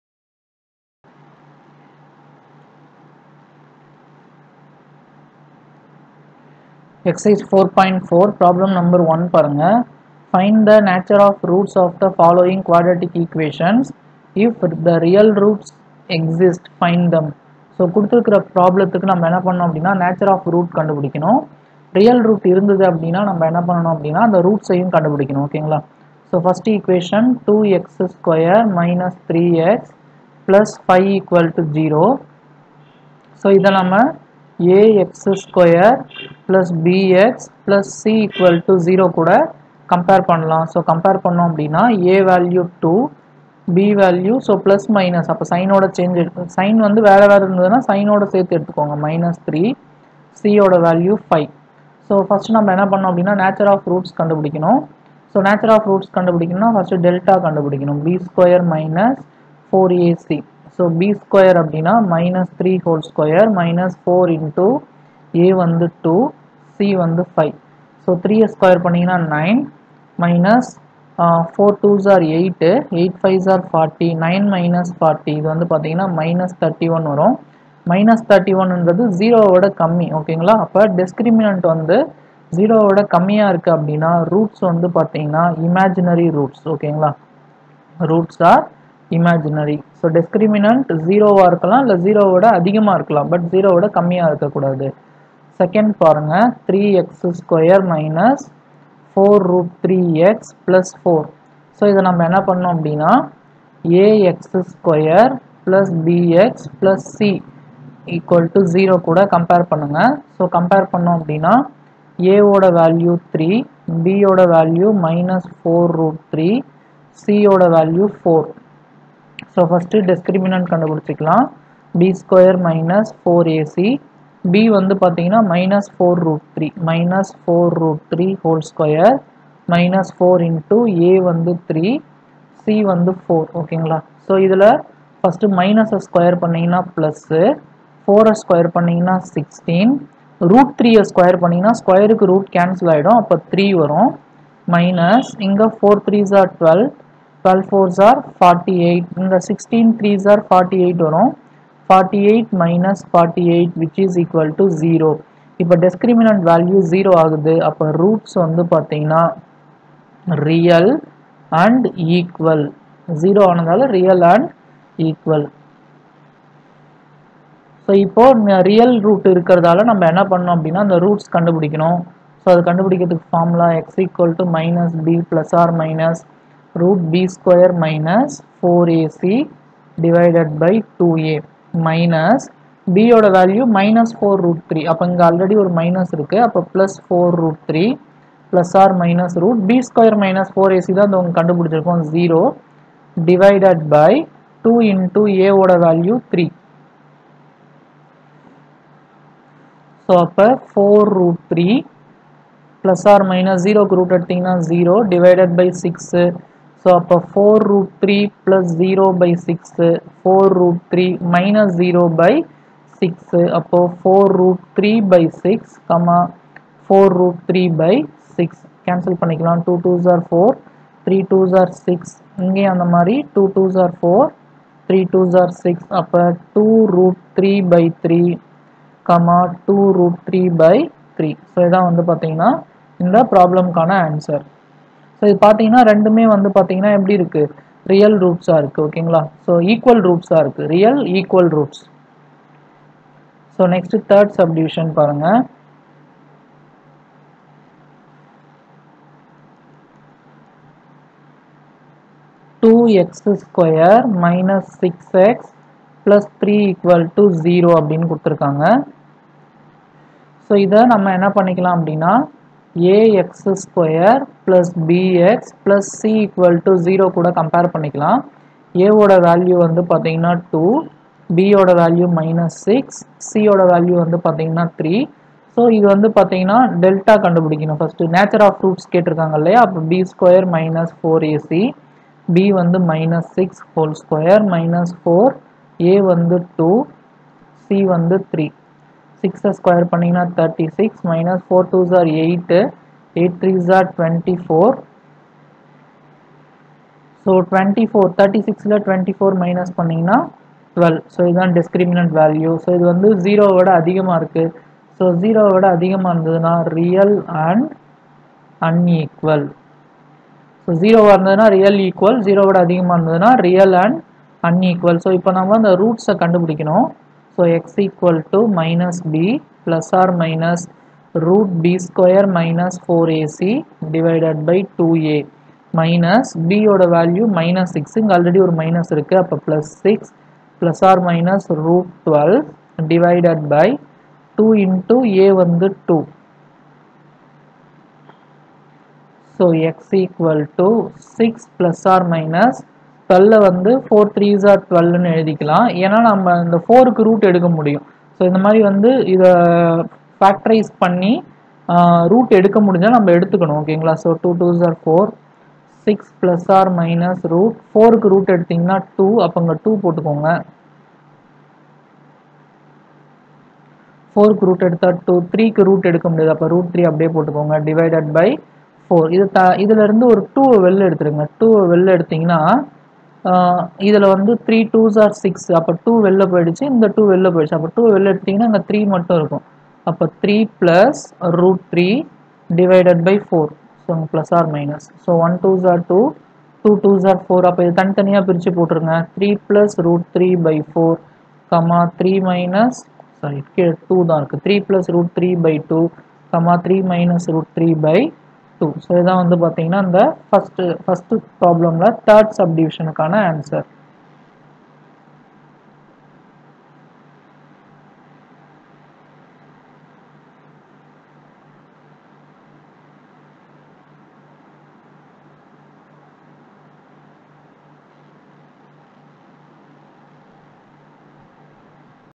Class 10, Maths, Exercise 4.4 Q. No. 1 in Tamil | Chapter 4 Quadratic Equations | CBSE
Topics:
4.1 Introduction
4.2 Quadratic Equations
4.3 Solution of a Quadratic Equation by Factorisation
4.4 Solution of a Quadratic Equation by Completing the Square
4.5 Nature of Roots